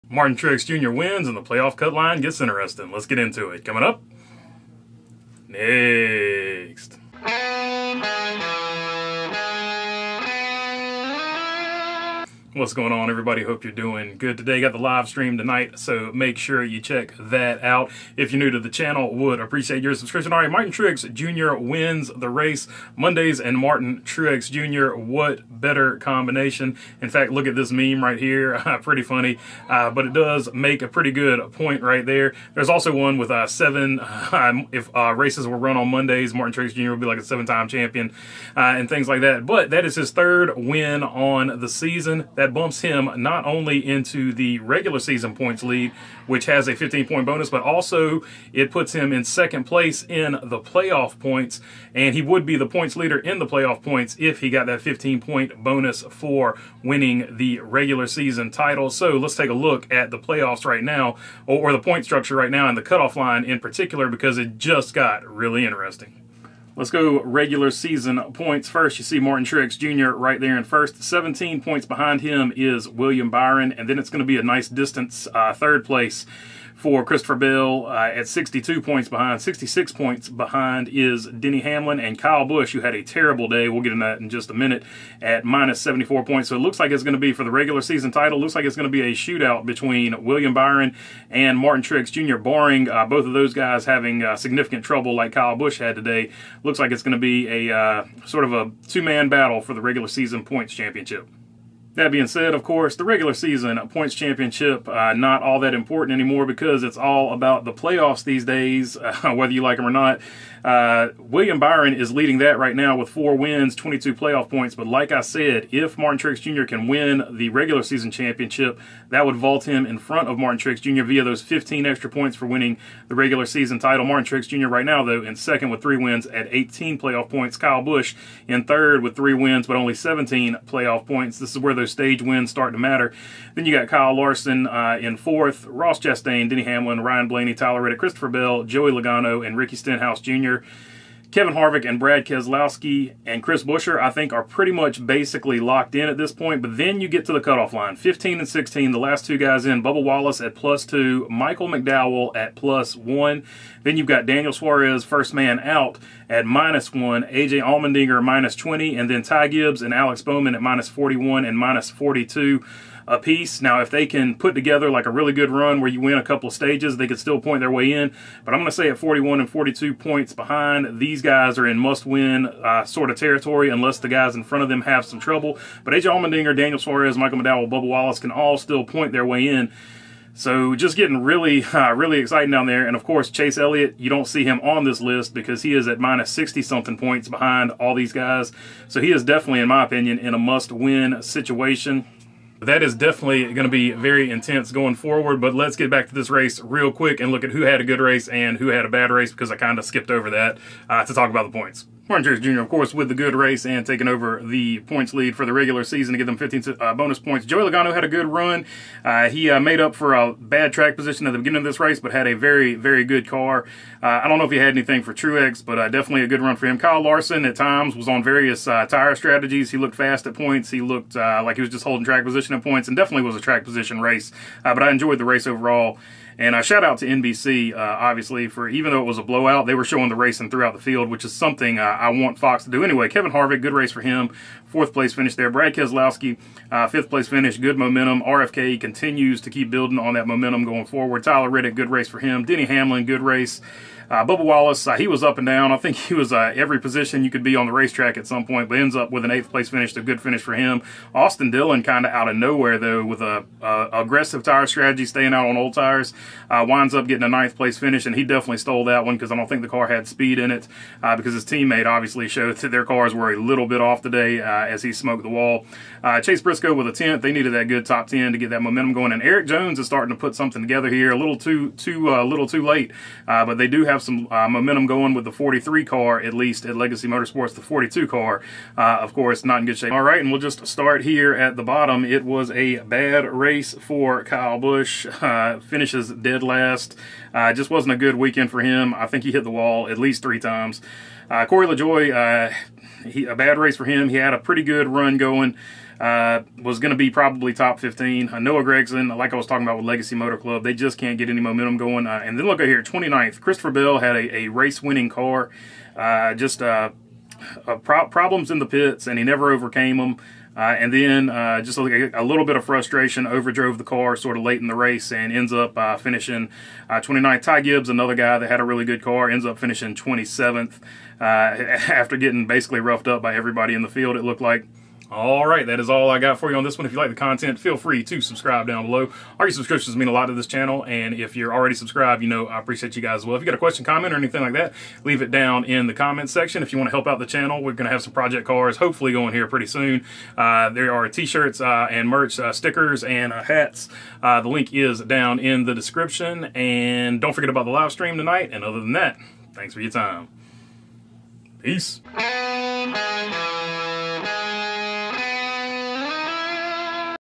Truex Wins Playoff Cut Line Gets Tight NASCAR New Hampshire News
Martin Truex dominates on a Monday like he does. His first win at New Hampshire in the cup series and he bites the Lobster. Chase Elliott is now in must win territory and Bubba Wallace, Daniel Suarez, Micheal McDowell, and Ty Gibbs among others like Alex Bowman Battle for the cut line for the NASCAR Cup Series championship. Bad Race for Kyle Busch as he finishes last leaving a two way battle almost for the regular season title between Byron and his Chevy Camaro and Martin Truex Jr and his Toyota Camry.